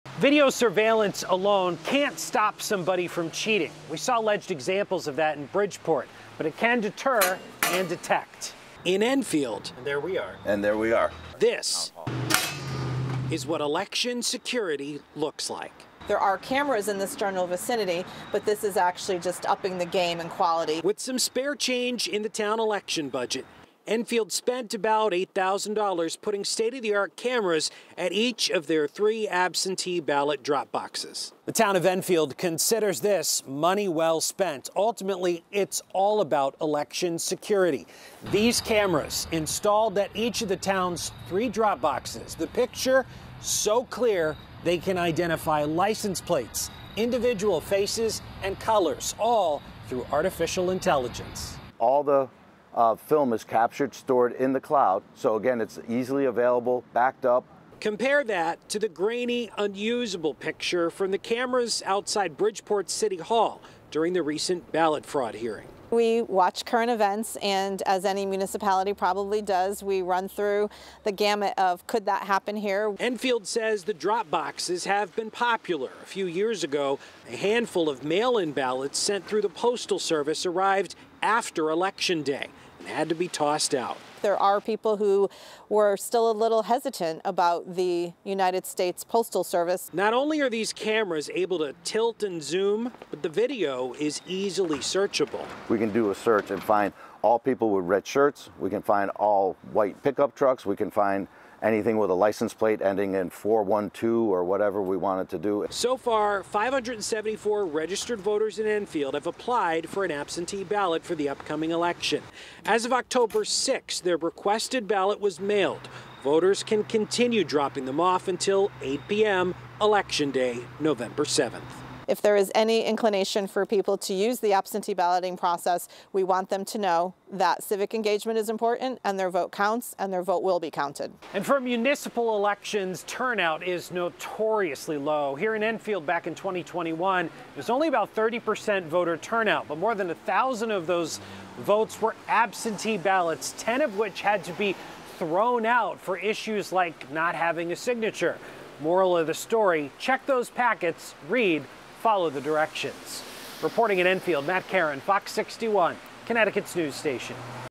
Enfield ballot boxes get security upgrade to curb election fraud
In the wake of the Bridgeport ballot battle, the town of Enfield is being proactive to make sure their drop boxes are as secure as possible.

Enfield has just installed state-of-the-art surveillance cameras above their three absentee ballot drop boxes. Video surveillance alone can’t stop someone from cheating. Alleged examples of that occurred in Bridgeport. But it can deter and detect.

"There are cameras in this general vicinity but this is just upping the game in quality," said Enfield Town Manager Ellen Zoppo-Sassu.

With some spare change in the town election budget, Enfield spent about $8,000 installing the cameras at the drop boxes outside Town Hall, Enfield Express and the library. The cameras are so clear they can identify and recognize faces and license plates.